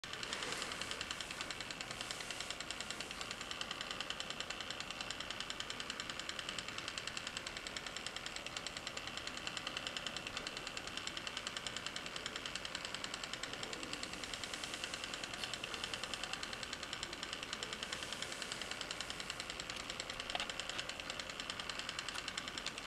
Spencer's Kinetic Clip 1
it's actually not supposed to move like this and is supposed to move like the spheres kinetic I uploaded, but I put it on my gemini base because I liked how it looks here better